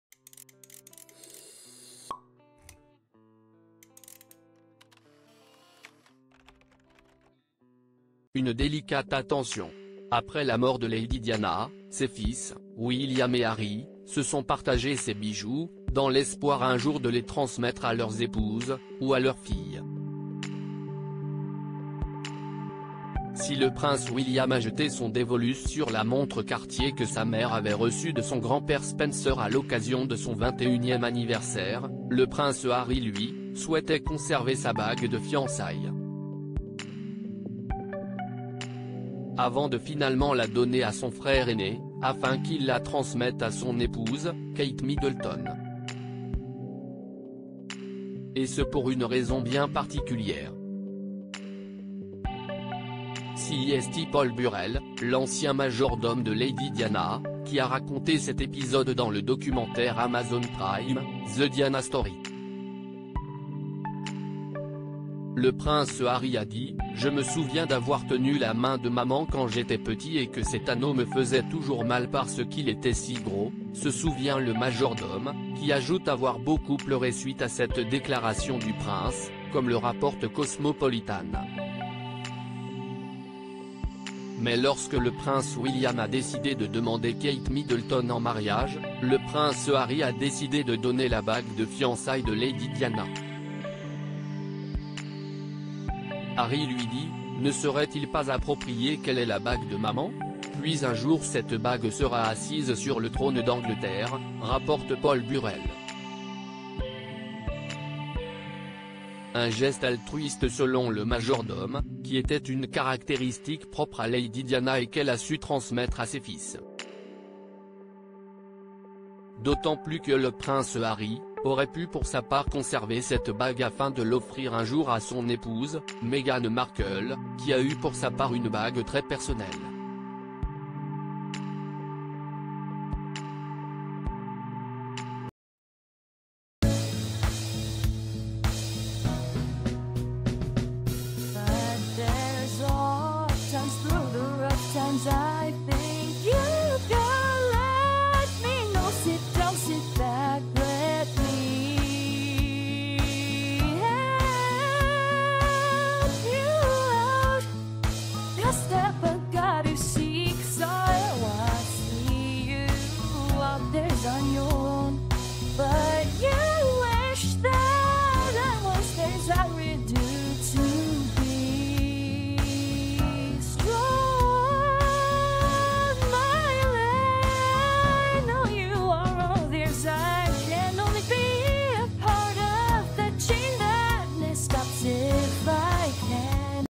Pourquoi le prince Harry a donné la bague de Lady Diana à son frère William lorsqu'il a demandé Kate
Pourquoi le prince Harry a donné la bague de Lady Diana à son frère William lorsqu'il a demandé Kate Middleton en mariage